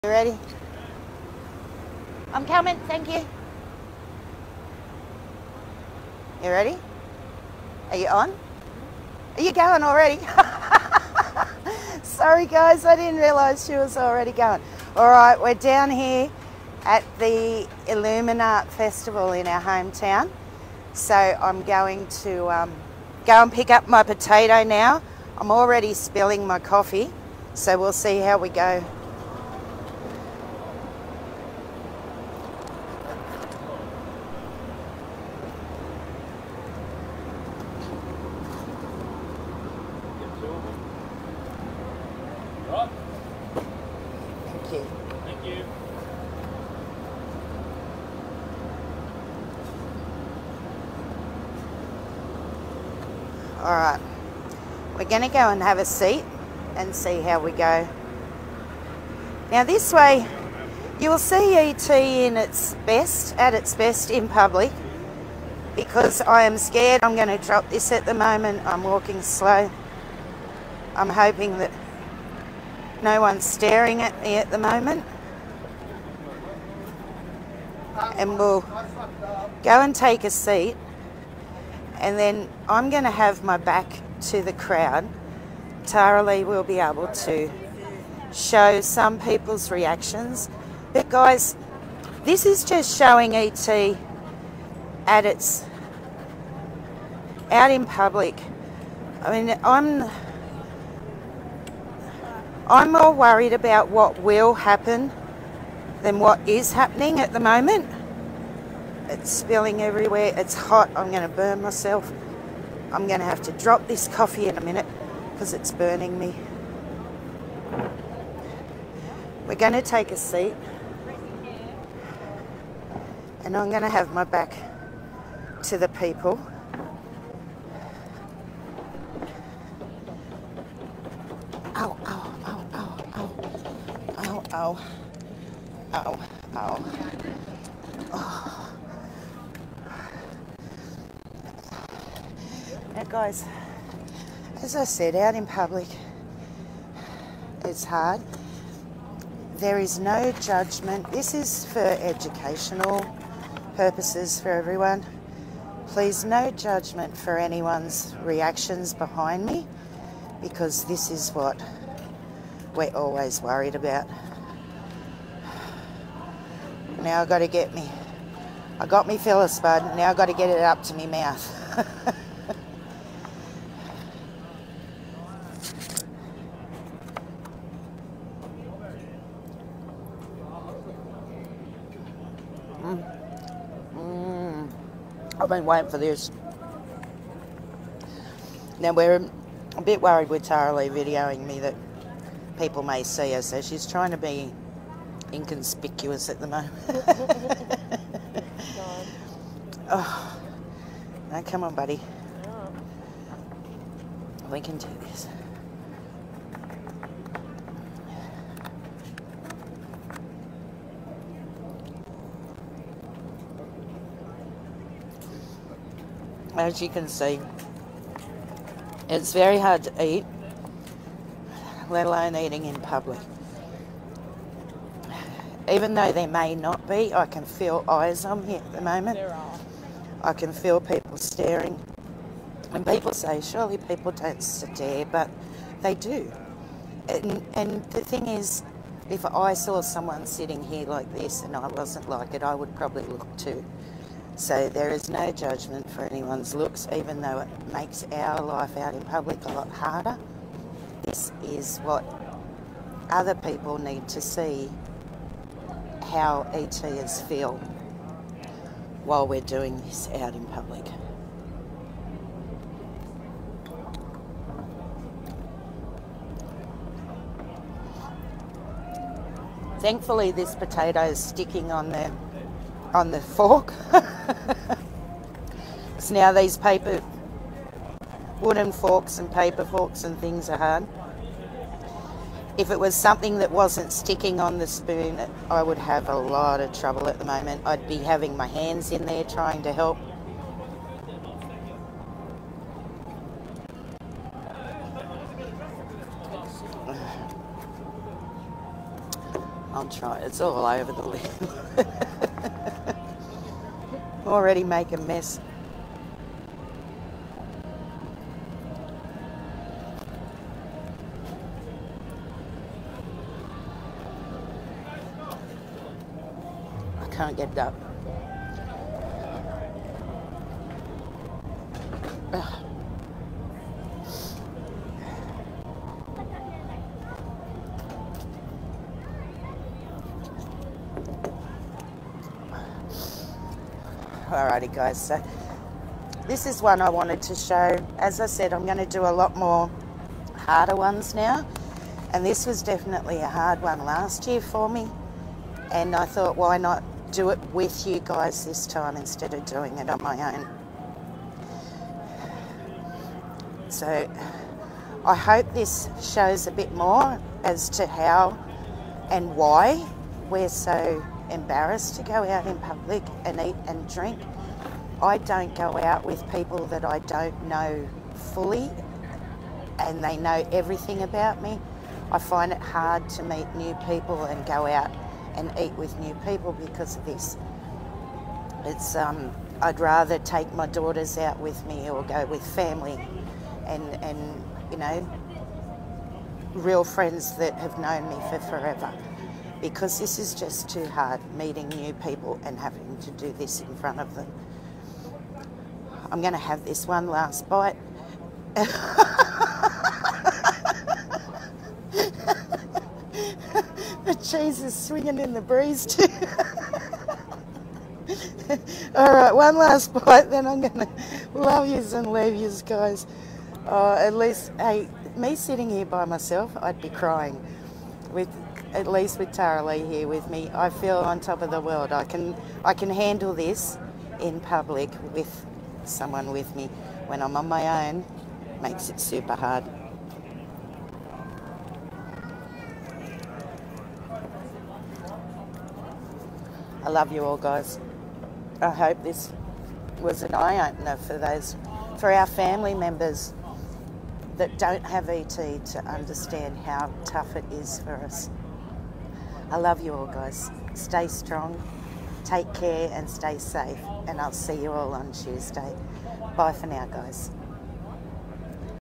Showing my ET while eating in public
Essential Tremor is hard enough to live with in our homes but eating and drinking in public is extremely hard.